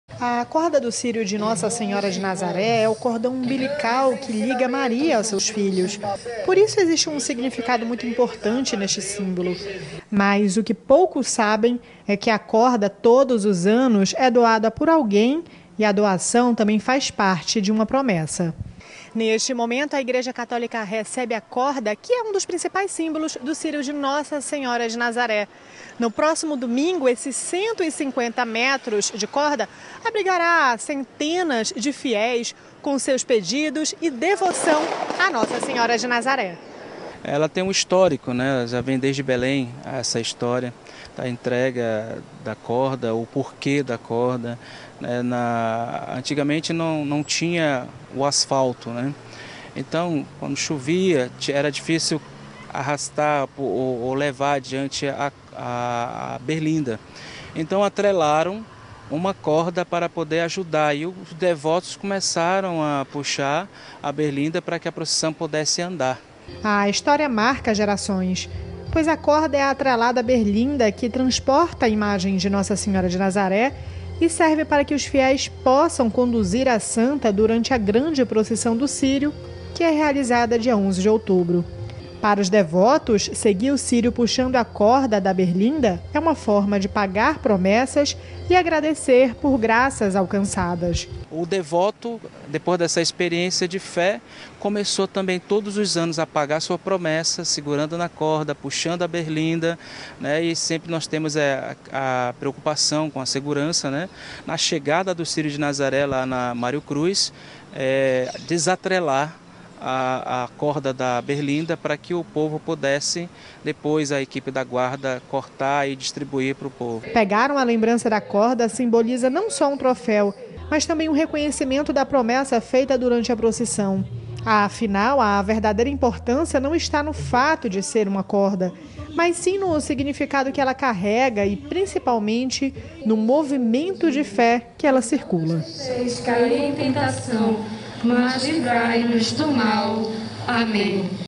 Amazônia News | Conheça o simbolismo da corda do círio de Nossa Senhora de Nazaré
Os significados da corda do Círio de Nazaré, Macapá